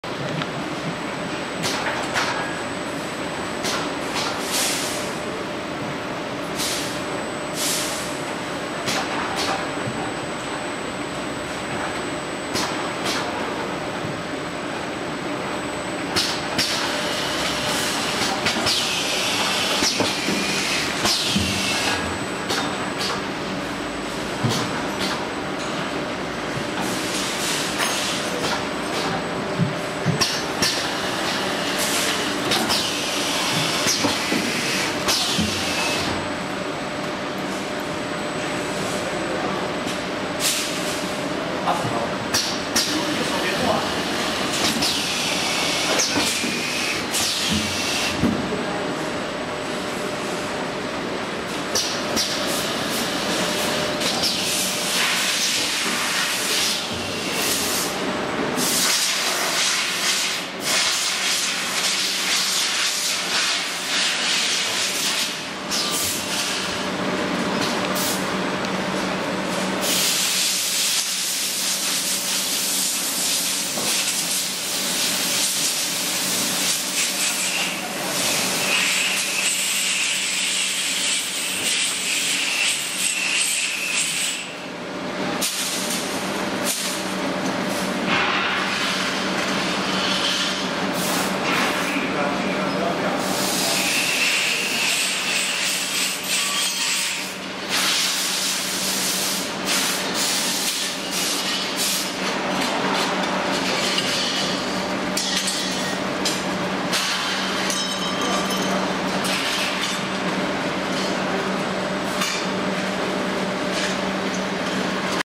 3x8 bottles shrink wrapping machine
One of my clients wanna know
00:00 Products feeding
00:08 automated sealing
00:22 hot shrink tunnel
00:37 shrink appearance